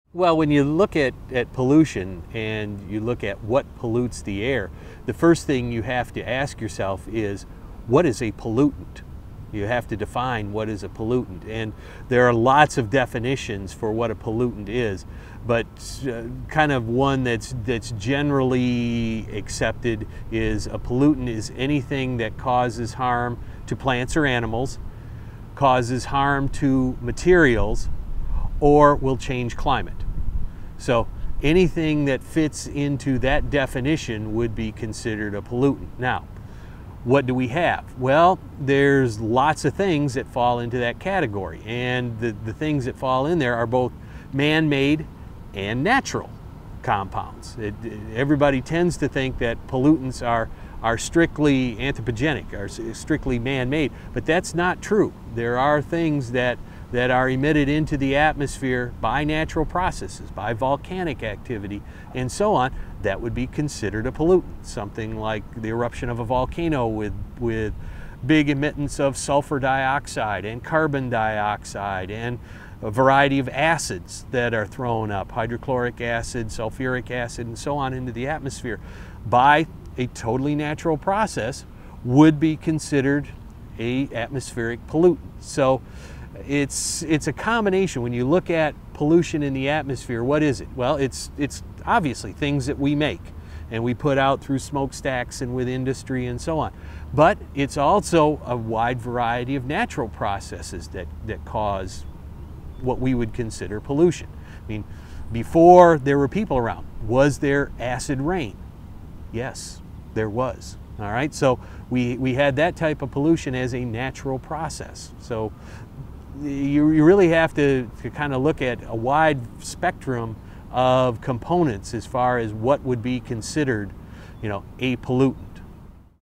Environmental Science & Weather : What Pollutes the Air?
Pollution in the air can be caused by substances that are both man-made and natural compounds. Find out how air pollution can be caused by volcanic eruptions with help from the chair of a department of environmental studies in this free video on air pollution.

Expert: Jack Hall
Bio: Dr. Jack Hall is the department chair in the department of environmental studies at the University of North Carolina at Wilmington.